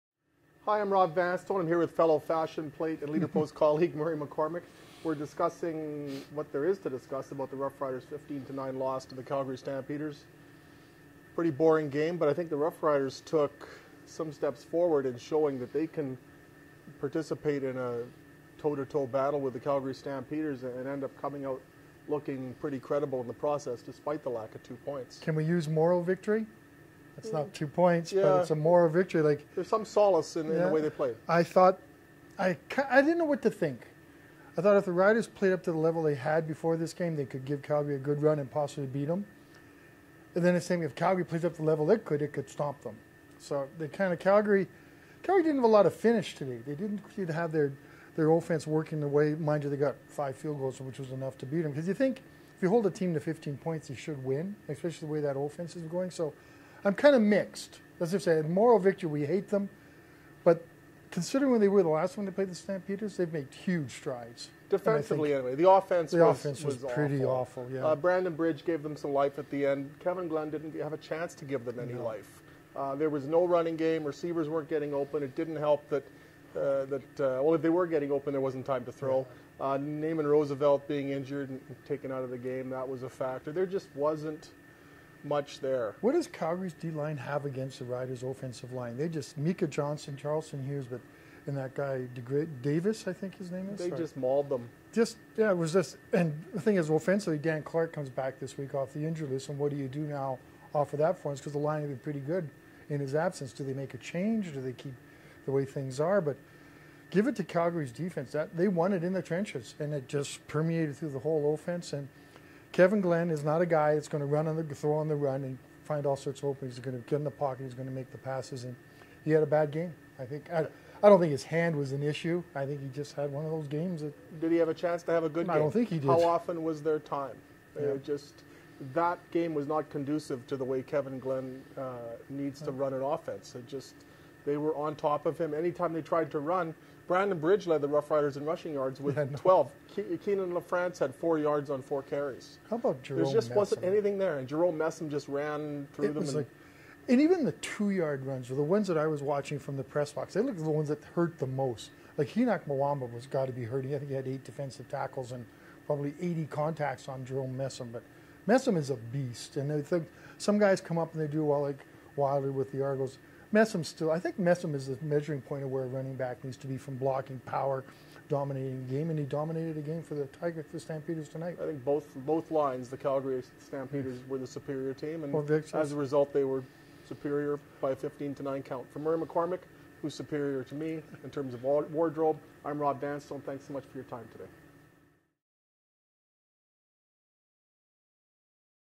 Rider Rumblings Sept. 24
Rob Vanstone and Murray McCormick discuss the game.